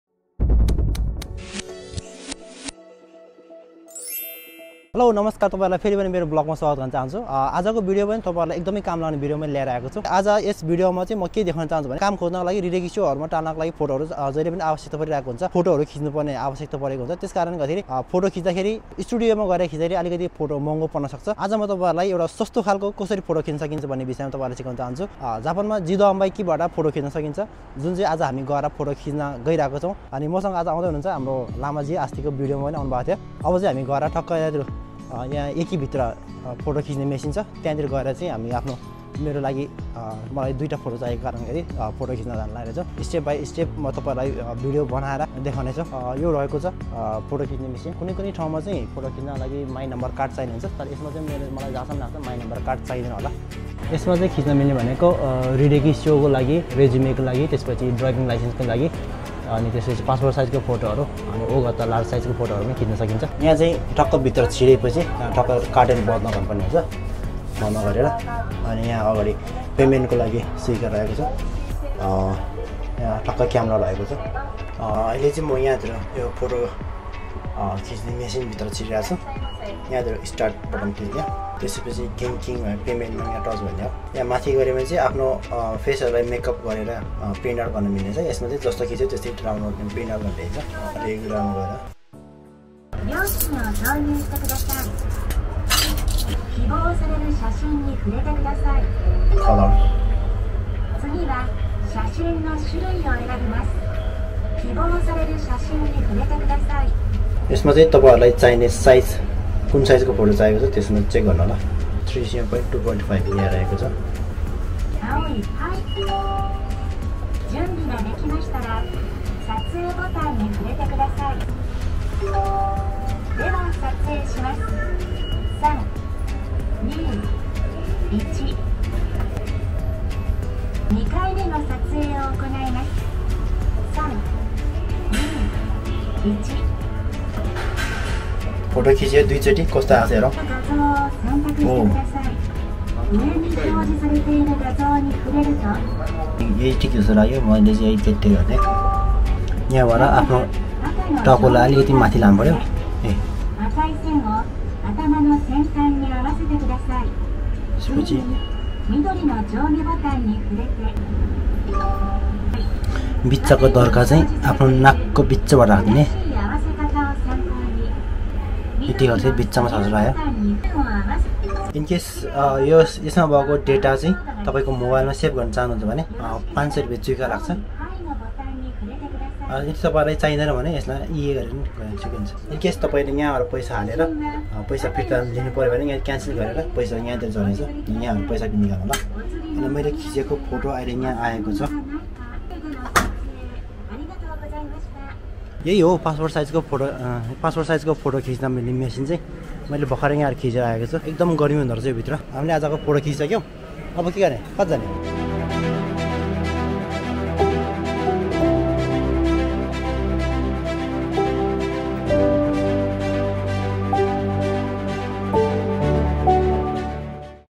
How To Shoot Passport Size Photo From Automatic Machine In Japan || Photos For 履歴書, Driving Licence
My Camera
My Hard Drive
My Laptop
My Gimble
My Mobile